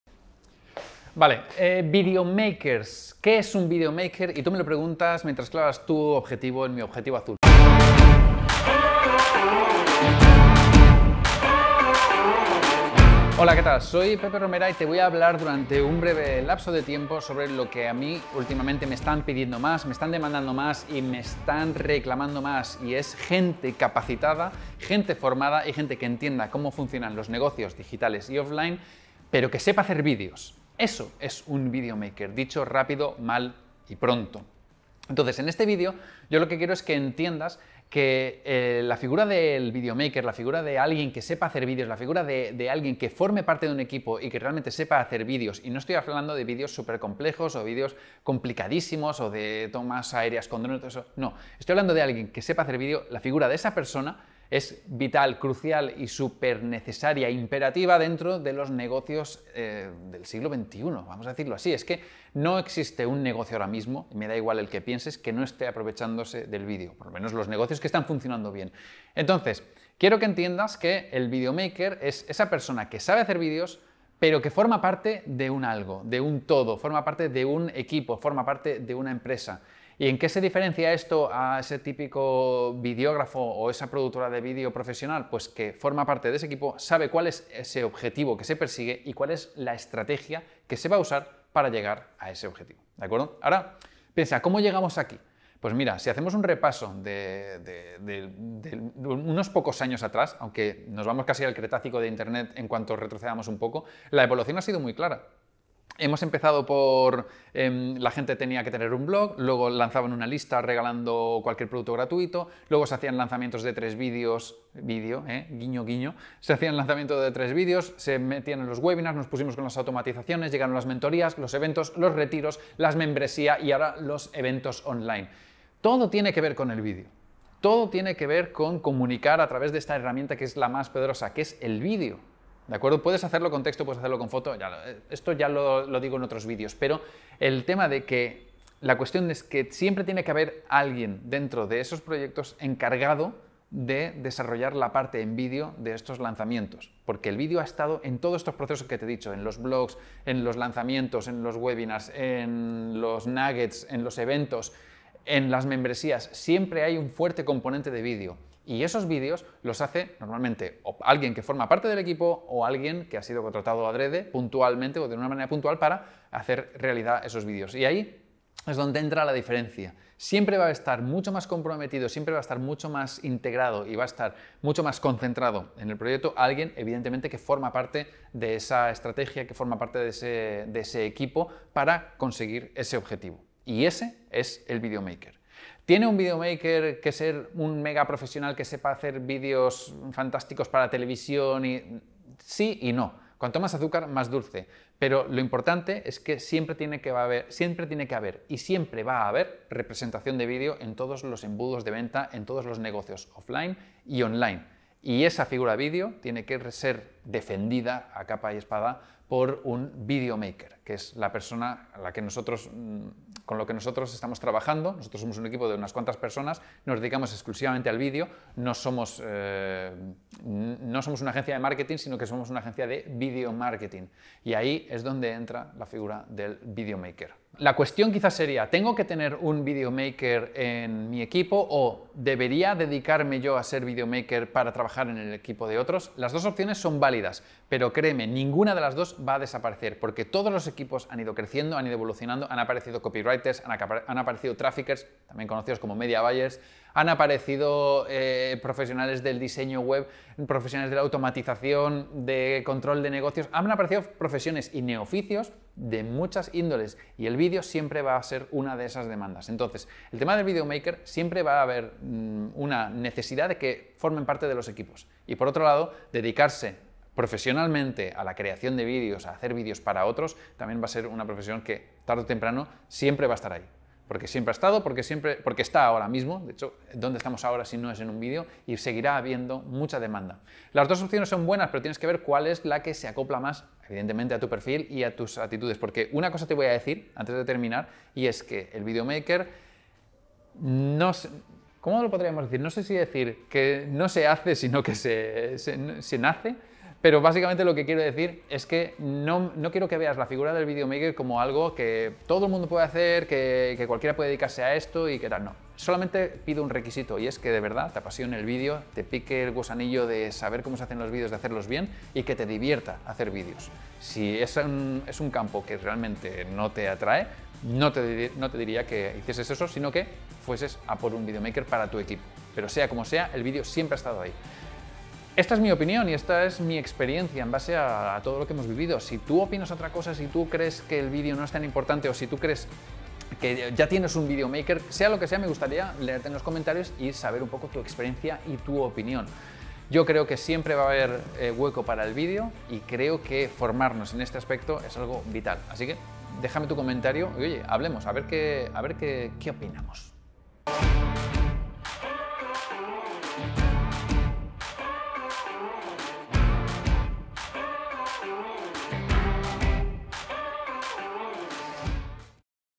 CURSO GRATIS: ¿Qué es un Vídeo maker? 🎬¿Gana MUCHO DINERO? 🎁¿Es chungo?
Te invito a un training gratuito en el que aprenderás cómo tú también puedes convertirte en Vídeo Maker

¿Sabes qué es un Vídeo Maker? El mismo nombre lo dice pero deberías ver el vídeo en vez de leer el resumen.

✅ El Vídeo Maker es la persona encargada de... (venga porfa, mira el vídeo que te lo explico muy bien y son solo 6 minutos, menos aún si lo pones a velocidad rápida). Conoce cuál es el objetivo que se persigue y domina la estrategia que se va a usar para conseguirlo, siempre usando el vídeo.

✅ El Vídeo Maker es un profesional como la copa de un pino, formado y preparado para entender lo que te cuento en la parte 3 del vídeo, pero que sabe hacer vídeos.

✅  Siempre han existido y siempre existirán los creadores de vídeo. El vídeo nunca va a desaparecer y los Vídeo Makers, tampoco. De hecho, es una de las profesiones con mayor demanda en la actualidad.

✅  Aunque la clave para ser Vídeo Maker es que te tiene que apasionar el vídeo, tienes que disfrutar haciendo vídeos. 💘🎬 A no ser que tengas mucha fuerza de voluntad y quieras hacerlo sin que te guste 🤮